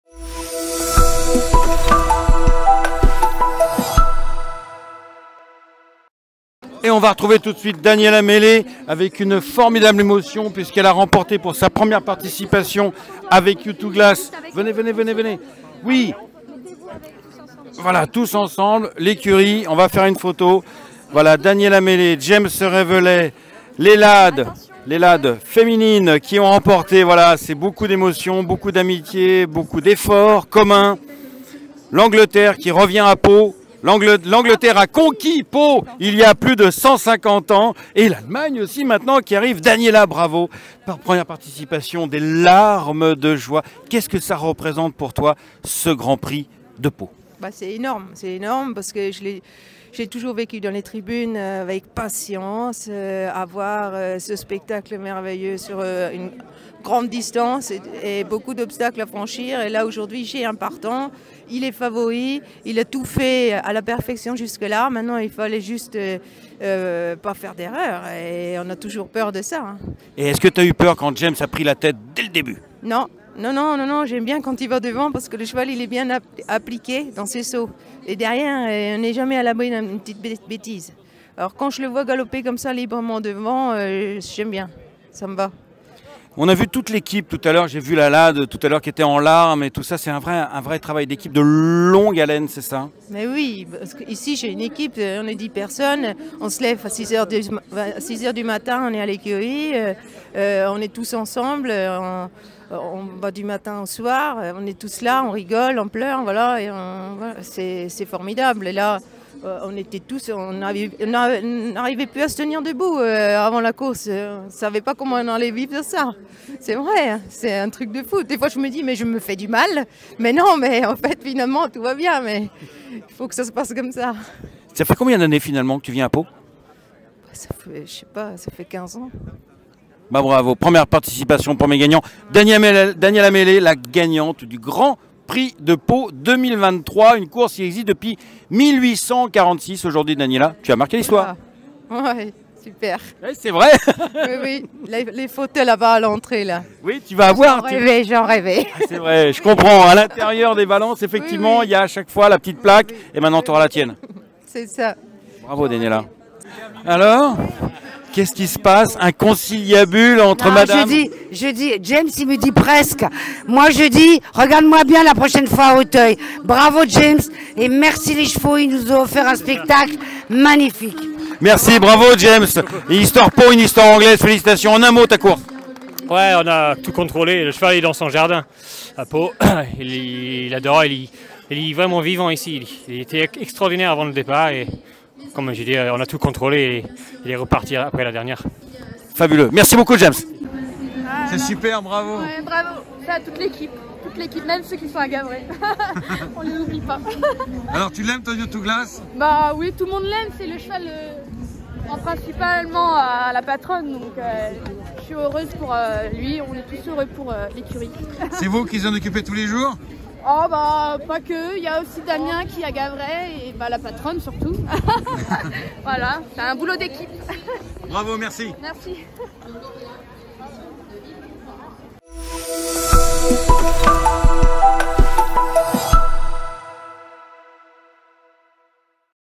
Grand Prix de Pau 2023: retour avec Daniela Mele et James Reveley après la victoire de Youtwo Glass
Découvrez les réactions à chaud de James Reveley, Daniela Mele et son équipe après la victoire de Youtwo Glass dans l'édition 2023 du Grand Prix de Pau (Gr.3), 136ème du nom.